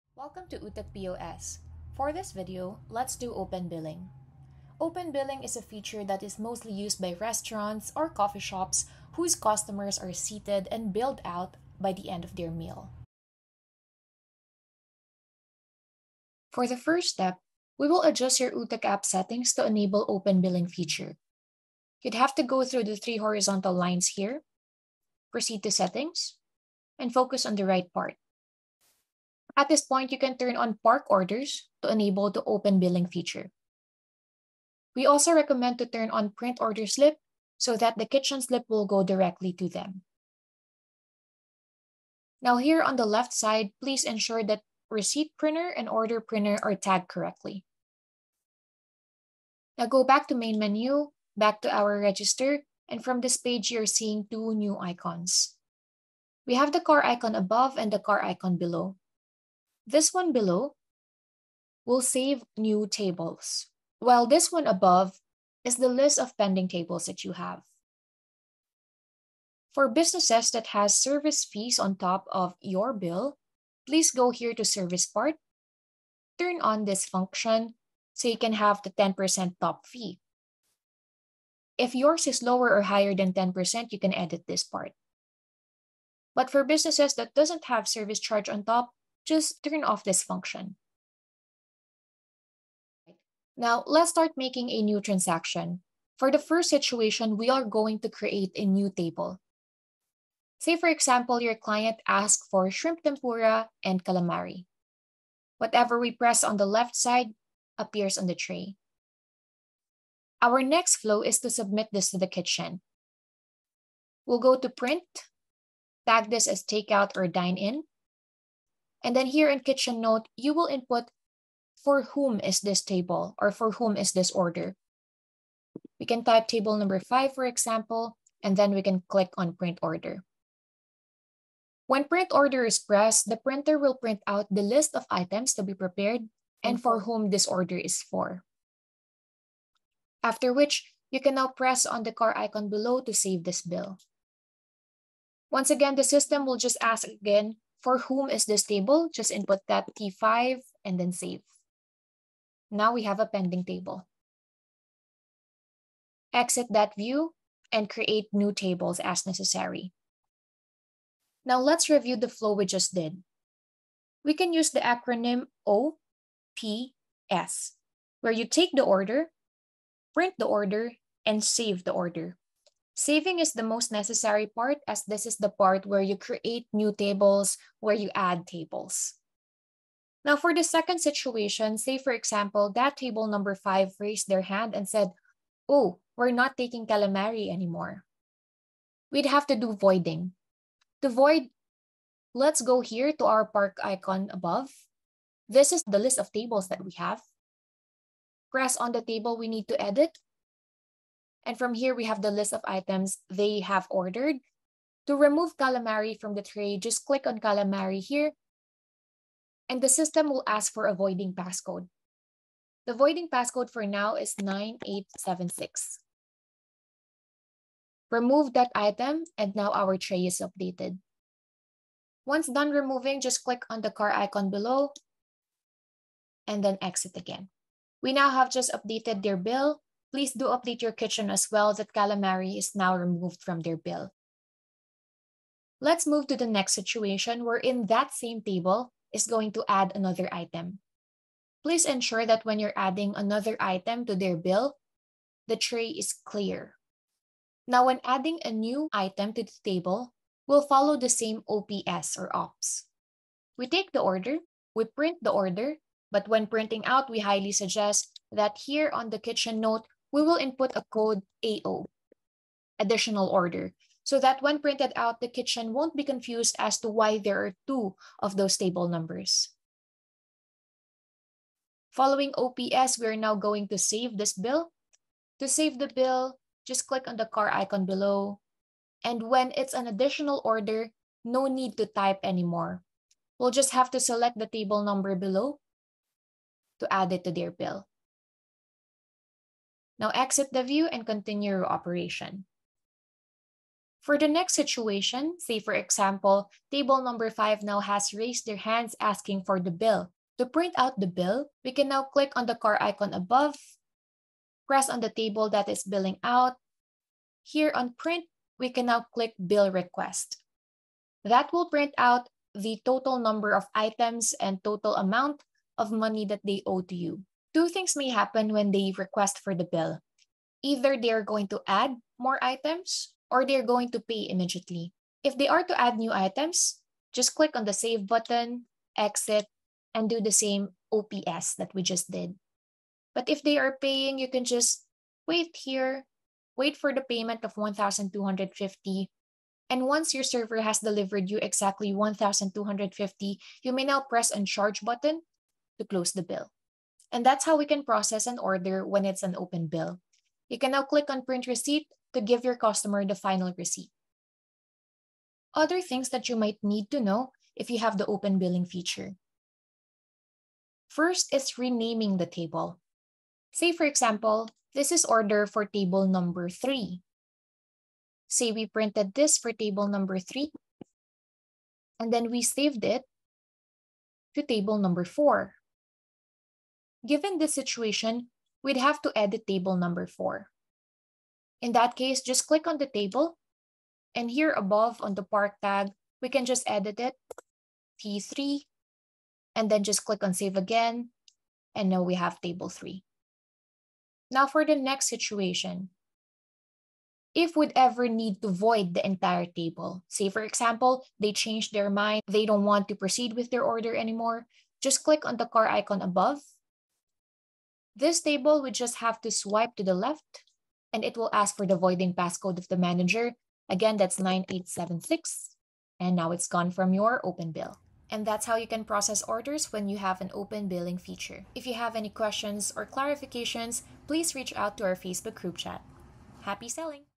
⚙️ How to Accept Open Billing
Parked Orders is a function in Utak POS to let your cashiers leave a tab open for a seated customer. This is usually used by restaurants and bistros when customers pay by the end of their meal. See this video to see how you can use this in your day-to-day operations!

00:00 - Introduction
00:20 - Utak POS Settings
00:50 - Icons on the Register View and Service Charge
01:40 - Creating a new table
03:00 - Voiding item from a table
04:00 - Adding an item to a table
04:53 - Billing Out
05:55 - Renaming a table
06:36 - Voiding/Deleting an entire table